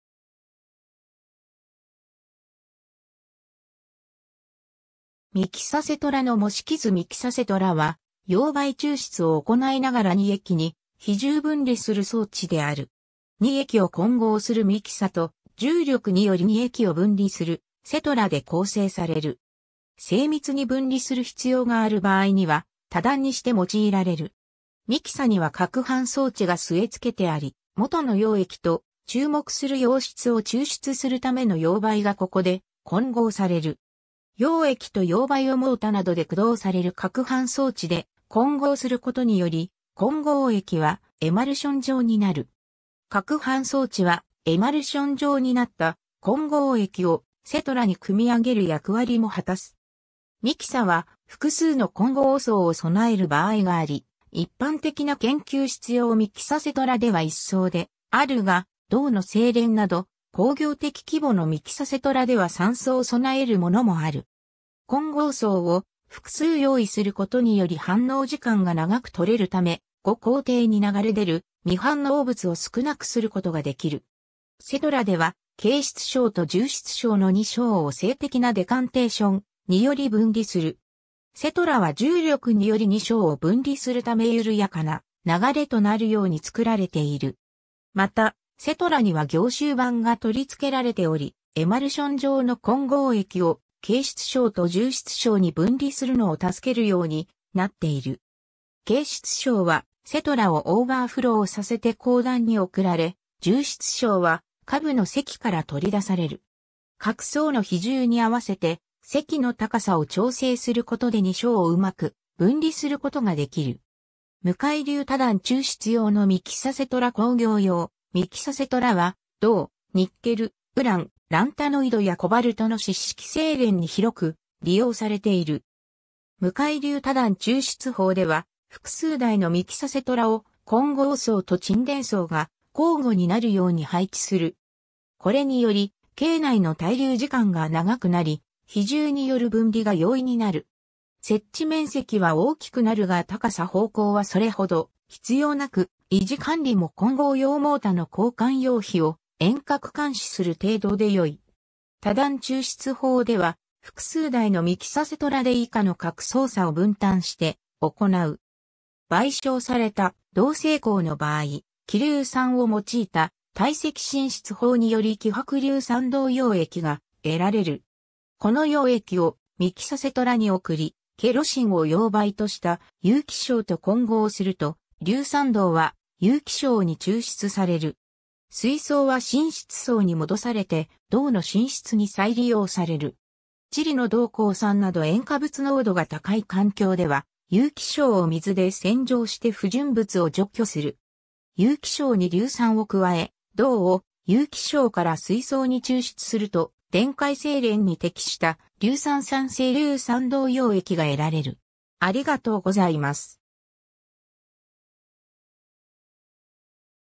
ミキサセトラ
ミキサセトラの模式図 ミキサセトラ（Mixer-settler）は溶媒抽出を行いながら2液に比重分離する装置である。
2液を混合するミキサ（混合槽）と、重力により2液を分離するセトラ（沈殿槽）で構成される。
精密に分離する必要がある場合には、多段にして用いられる。
 ミキサ（混合槽）には攪拌装置が据え付けてあり、元の溶液と、注目する溶質を抽出するための溶媒がここで混合される。
溶液と溶媒をモータなどで駆動される攪拌装置で混合することにより、混合液はエマルション状になる。
攪拌装置はエマルション状になった混合液をセトラに汲み上げる役割も果たす。
ミキサは複数の混合槽を備える場合があり、一般的な研究室用ミキサセトラでは1槽であるが、銅の精錬など工業的規模のミキサセトラでは3槽備えるものもある。
混合槽を複数用意することにより反応時間が長く取れるため、後工程に流れ出る未反応物を少なくすることができる。
 セトラ（沈殿槽）では軽質相と重質相の2相を静的なデカンテーションにより分離する。
セトラは重力により2相を分離するため緩やかな流れとなるように作られている。
また、セトラには凝集板が取り付けられており、エマルション状の混合液を軽質相と重質相に分離するのを助けるようになっている。
軽質相はセトラをオーバーフローさせて後段に送られ、重質相は下部の堰から取り出される。
各相の比重に合わせて堰の高さを調整することで2相をうまく分離することができる。
 向流多段抽出用のミキサセトラ（4段構成の例） 工業用ミキサセトラは銅、ニッケル、ウラン、ランタノイドやコバルトの湿式製錬に広く利用されている。
 向流多段抽出法では、複数台のミキサセトラを混合槽と沈殿槽が交互になる（沈殿槽の出側を次段の混合層の入側になる）ように配置する。
これにより、系内の滞留時間が長くなり、比重による分離が容易になる。
設置面積は大きくなるが高さ方向はそれほど必要なく、維持管理も混合用モータの交換要否を遠隔監視する程度でよい(Colven, 1956; Davidson, 1957年)。
 多段抽出法では、複数台のミキサセトラで以下の各操作を分担して行う。
 焙焼された銅精鉱の場合、希硫酸を用いた堆積浸出法により希薄硫酸銅(II)溶液が得られる。
この溶液をミキサセトラに送り、ケロシンを溶媒とした有機相と混合すると、硫酸銅(II)は有機相に抽出される。
水相は浸出槽に戻されて銅の浸出に再利用される。
 チリの銅鉱山など塩化物濃度が高い環境では、有機相を水で洗浄して不純物を除去する。
 有機相に硫酸を加え、銅を有機相から水相に抽出すると、電解精錬に適した硫酸酸性硫酸銅(II)溶液が得られる。